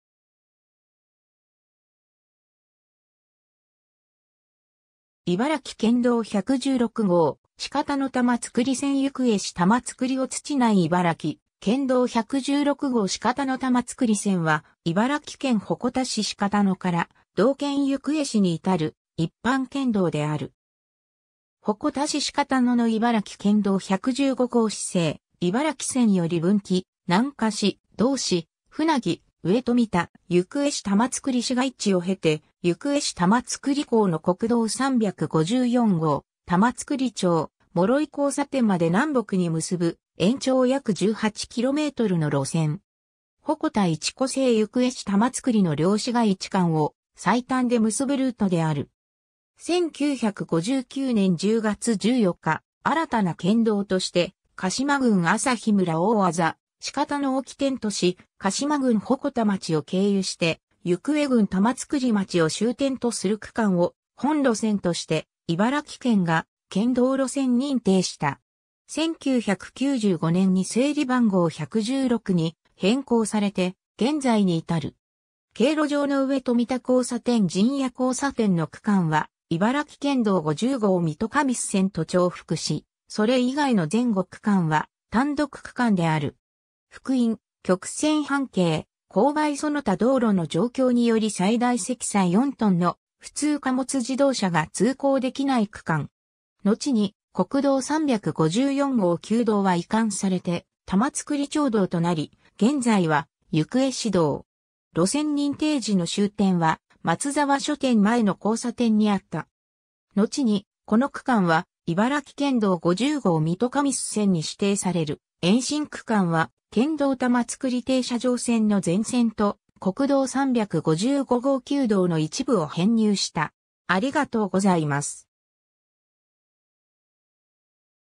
茨城県道116号鹿田玉造線
茨城県道116号鹿田玉造線 行方市玉造乙地内（2013年4月） 茨城県道116号鹿田玉造線（いばらきけんどう116ごう しかだたまつくりせん）は、茨城県鉾田市鹿田から同県行方市に至る一般県道である。
 鉾田市鹿田の茨城県道115号子生茨城線より分岐・南下し、同市舟木、上冨田、行方市玉造市街地を経て、行方市玉造甲の国道354号・玉造町諸井交差点まで南北に結ぶ延長約18 kmの路線。
鉾田市子生 - 行方市玉造の両市街地間を最短で結ぶルートである。
 1959年（昭和34年）10月14日、新たな県道として鹿島郡旭村大字鹿田を起点とし、鹿島郡鉾田町を経由して行方郡玉造町を終点とする区間を本路線として茨城県が県道路線認定した。
1995年（平成7年）に整理番号116に変更されて現在に至る。
 経路上の上冨田交差点 - 陣屋交差点の区間は、茨城県道50号水戸神栖線と重複し、それ以外の前後区間は単独区間である。
 ^ のちに国道354号旧道は移管されて玉造町道となり現在は行方市道。
路線認定時の終点は、松沢書店前の交差点にあった。
 ^ のちに、この区間は茨城県道50号水戸神栖線に指定される ^ 延伸区間は、県道玉造停車場線の全線（重複）と国道355号旧道の一部を編入した。